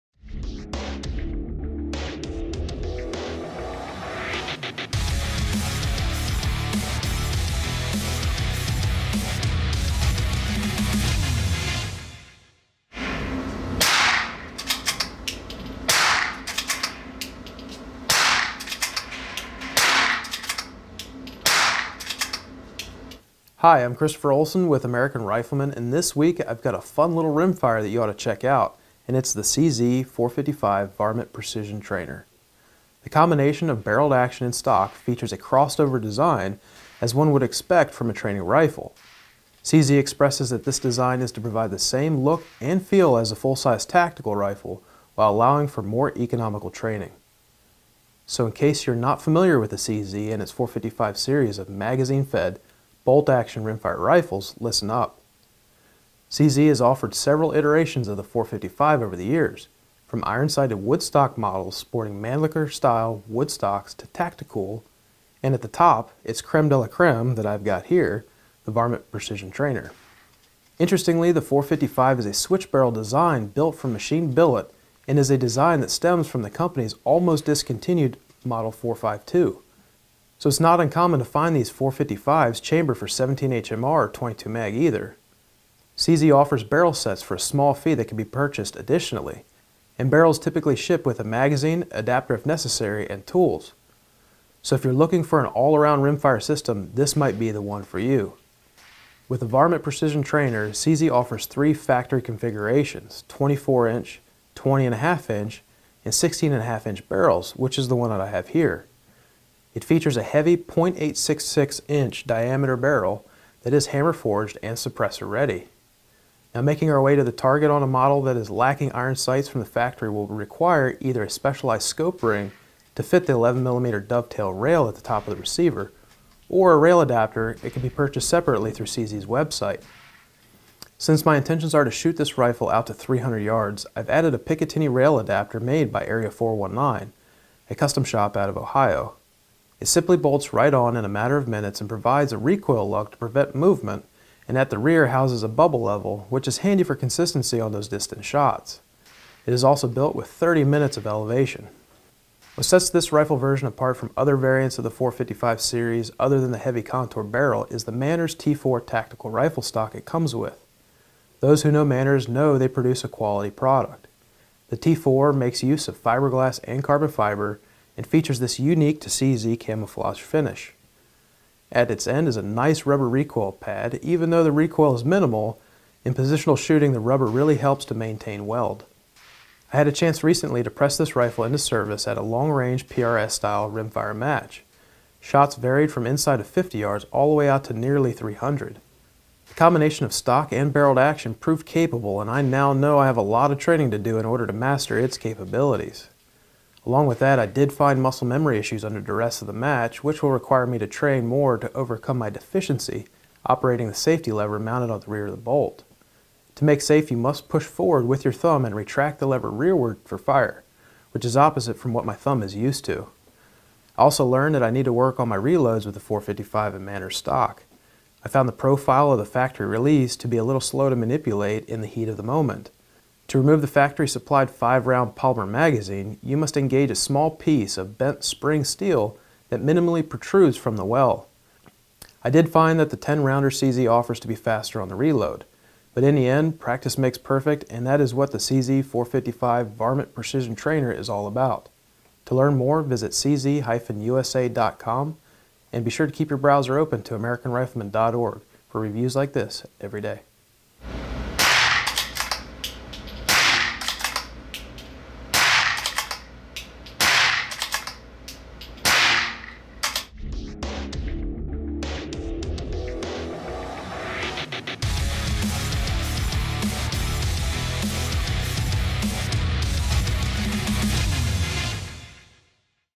NRA Gun of the Week: CZ 455 Varmint Precision Trainer Camo Rifle
American Rifleman's Christopher Olsen reviews a rimfire trainer from CZ.